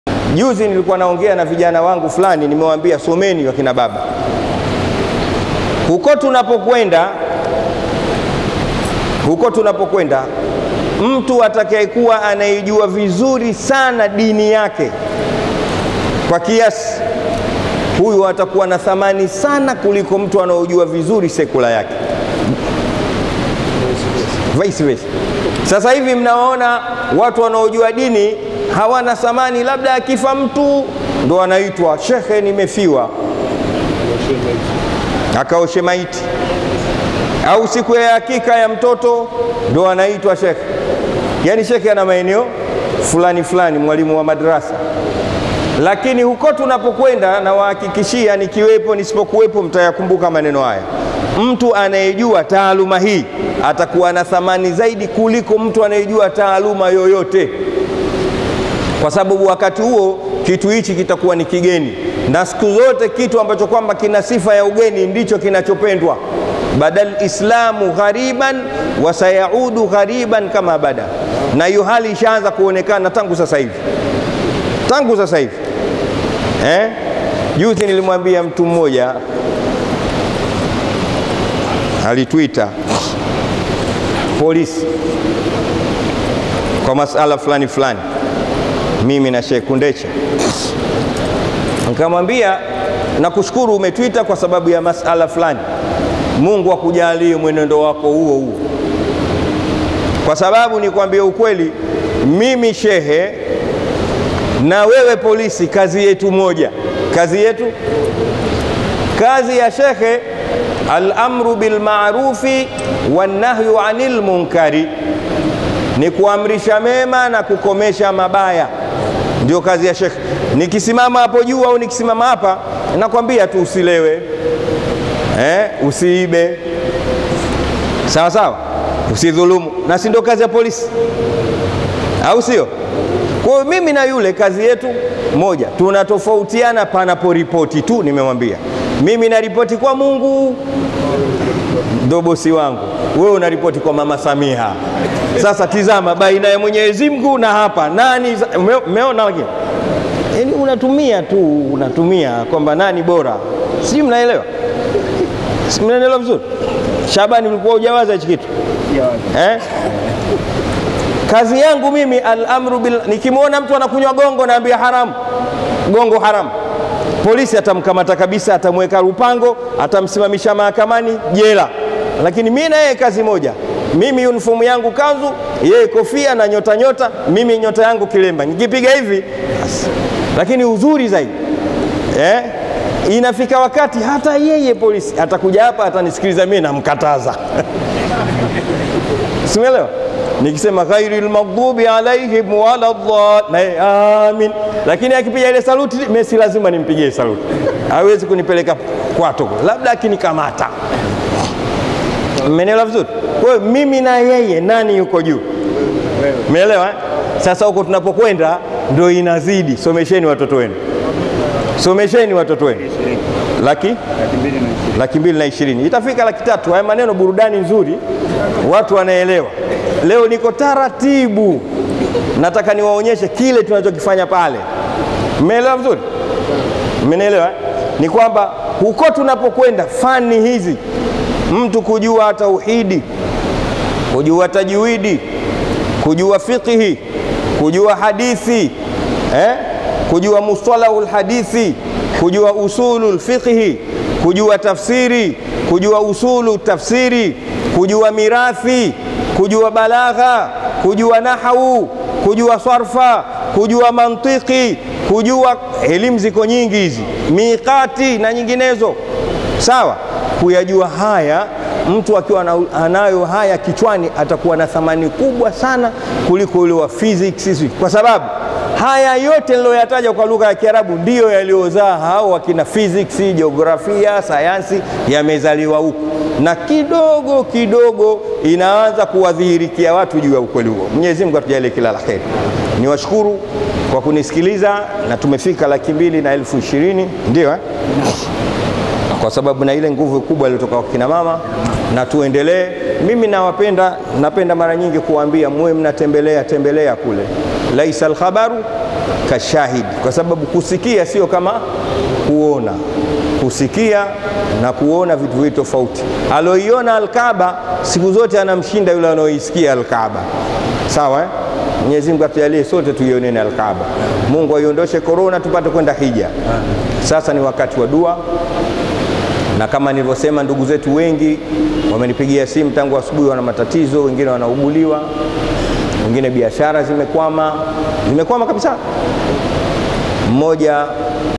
Kazi Ya Sheikh Na Police Ni Moja / Somesheni Watoto Wenu Dini / Sheikh Walid Alhad
Kazi Ya Sheikh Na Police Ni Moja Tofauti  Ni Moja Tu / Somesheni Watoto Wenu  Dini / Sheikh Walid Alhad

Mashaa Allah Bingwa sheikh Walid Alhad Omari akieleza kwa uzur na ubora wa hali ya juu kabisa juu ya Kazi ya Sheikh na police ni moja na tofauti yake kwenye kuripoti  tizama video zetu  mpaka mwisho usisahau kushare

And
Thanks